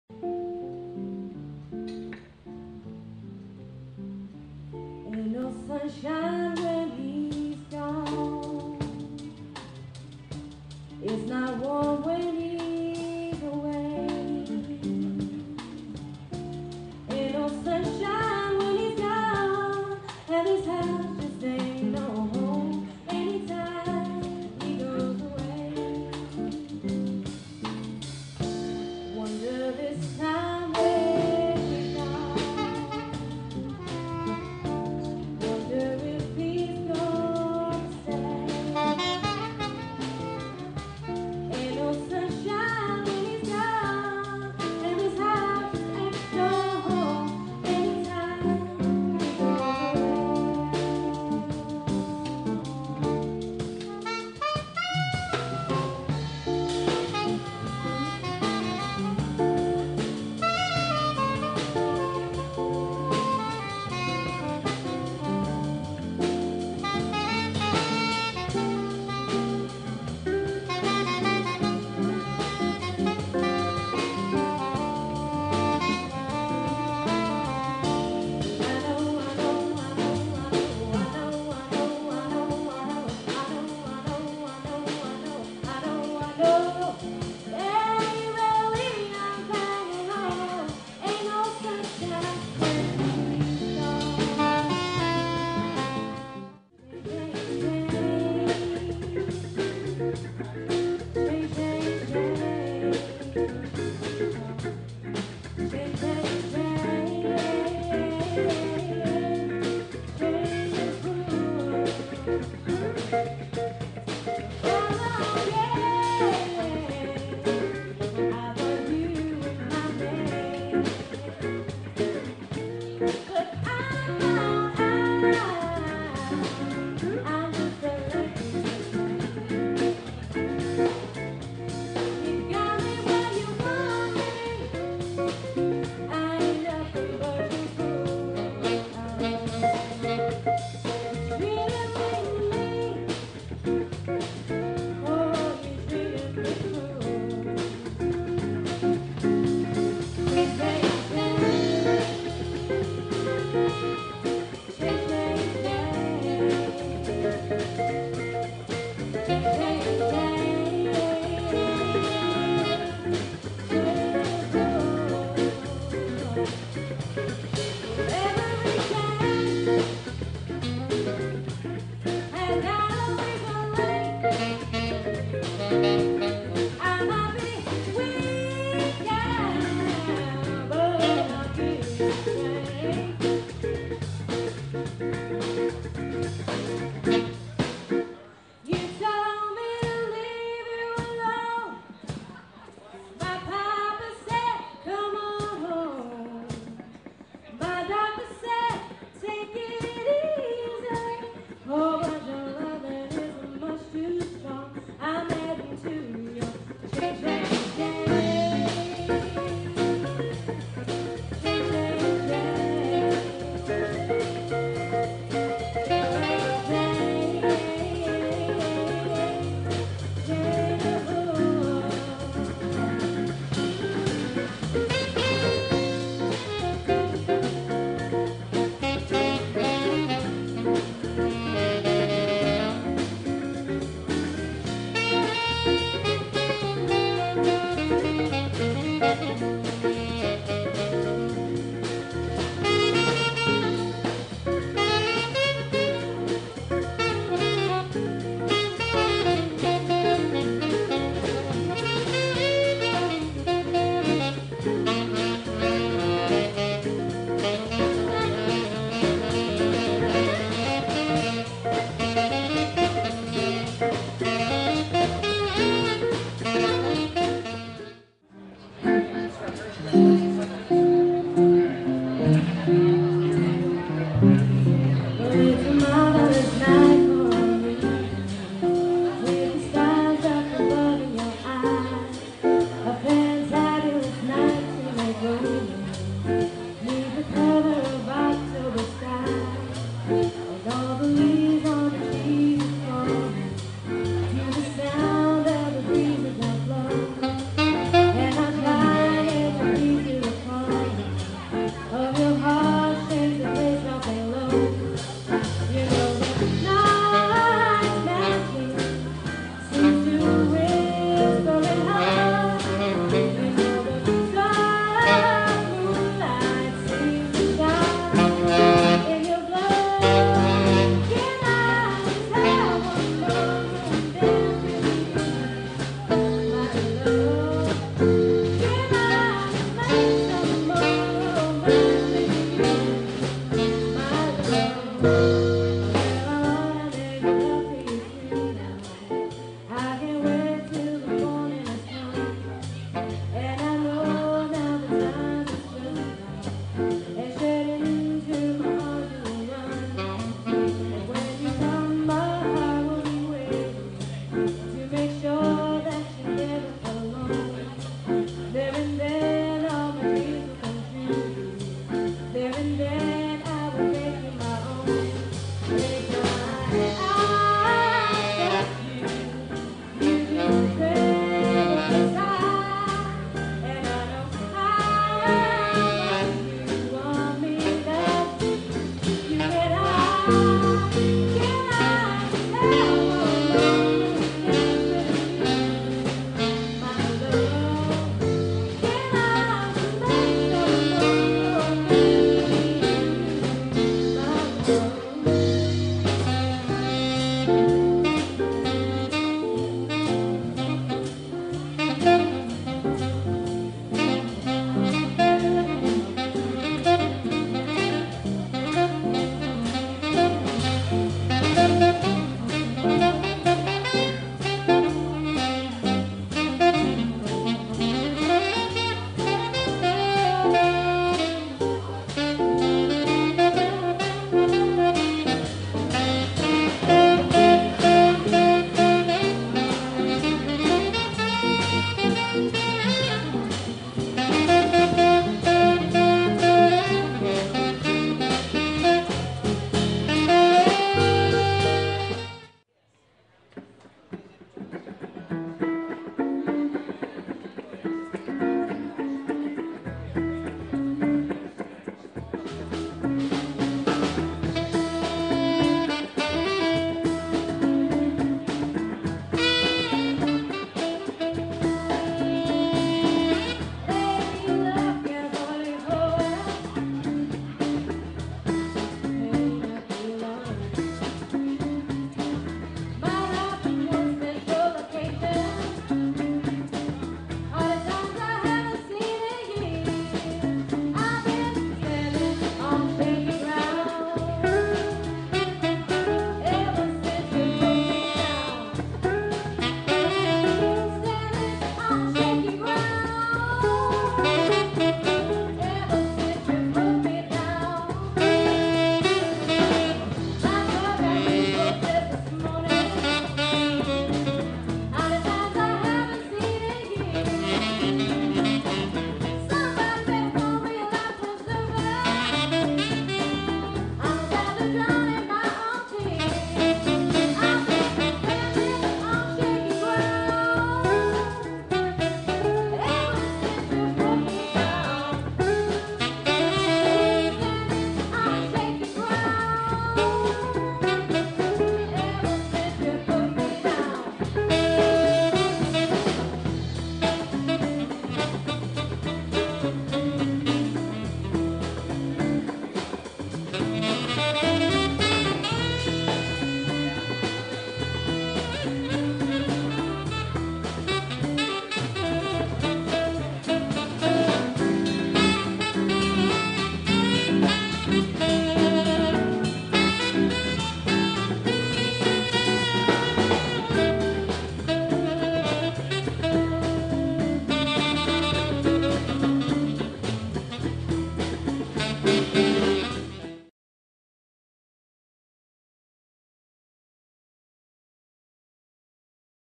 SaxophoneJack Quartet with Ashley Bean
Providing live music at Baker Peters Jazz Club in Knoxville, TN in July of 2010.  Great outside venue!